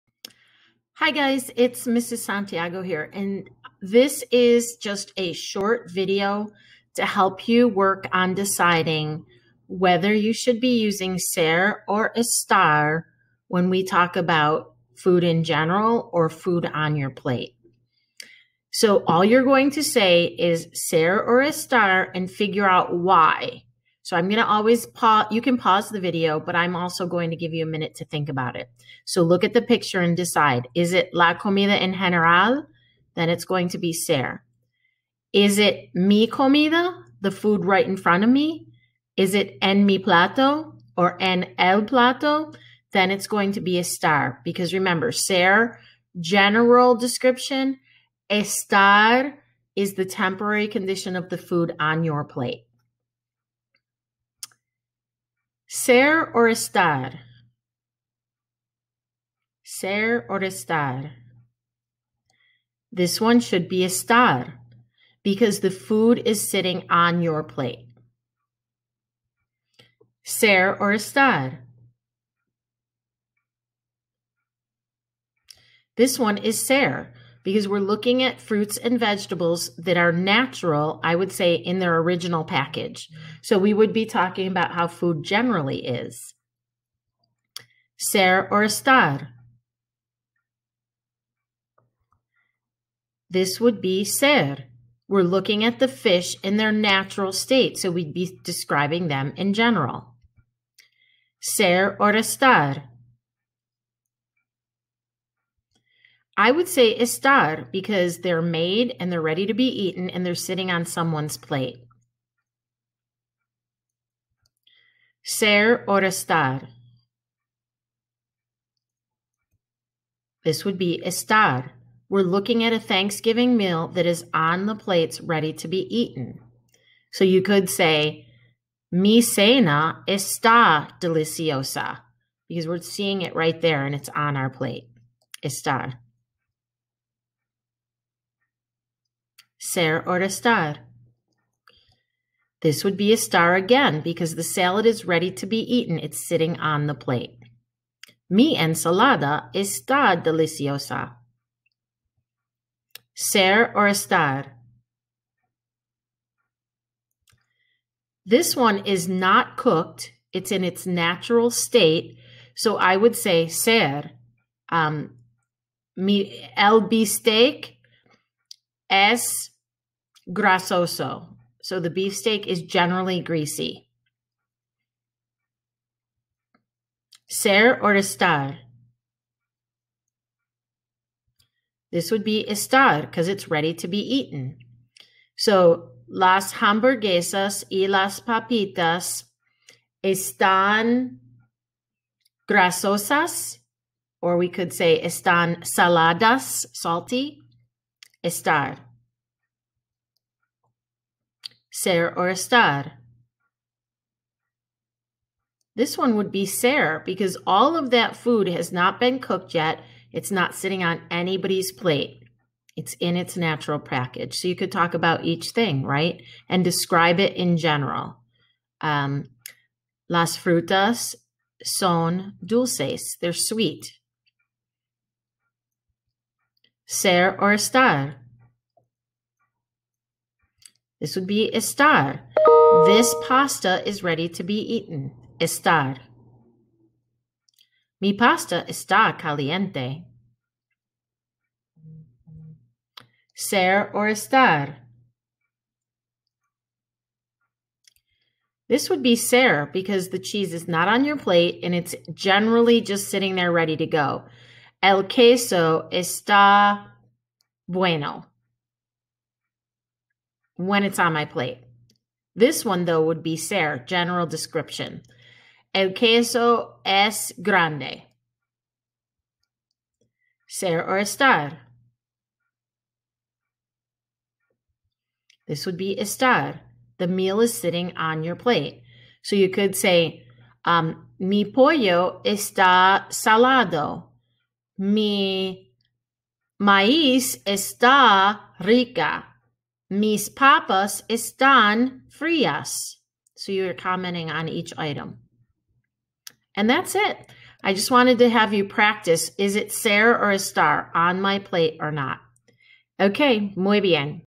ser o estar con comida con Sra Santiago
Looking at food to determine - would you use ser or estar to talk about that food?